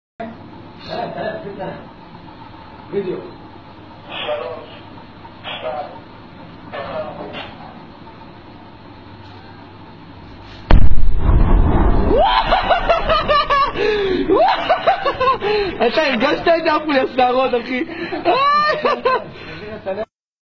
Israel Diaper Force bombing Mosque
Israel Defense Force excitingly bombing Mosque in Gaza. You can hear from their voice talking in Hebrew how proudly they are bombing mosque. There are civilians inside the mosque how could you just heartlessly destructing civilian building.